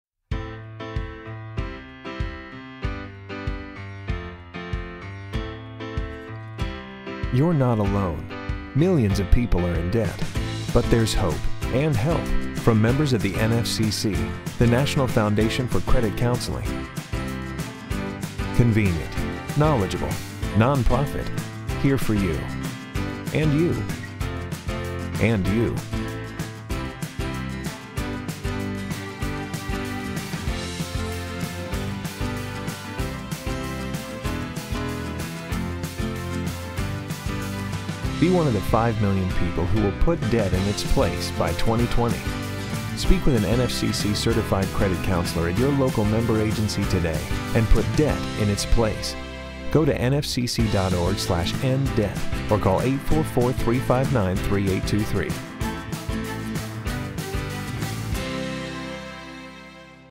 NFCC You're Not Alone PSA
The National Foundation for Credit Counseling Member Agencies have helped millions of people just like you reach their financial goals. As the nation’s largest and longest-serving financial counseling organization, NFCC Certified Consumer Credit Counselors stand ready to do the same for you.